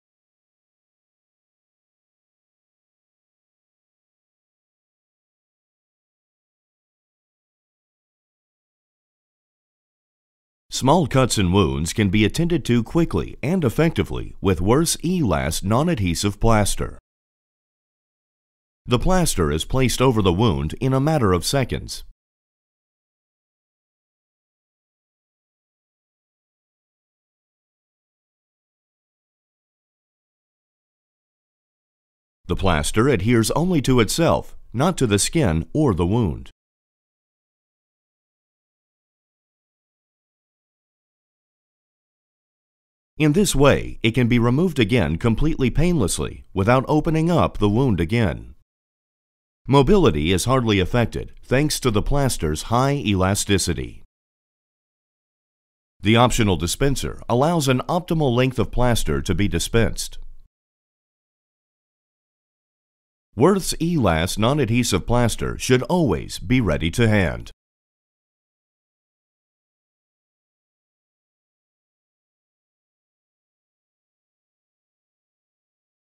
Límlaus plástur
Límlaus plástur sem auðvelt er að móta á hvern hátt sem er.
Einstaklega þægilegur þar sem ekkert lím situr eftir þegar plástur er fjarlægður.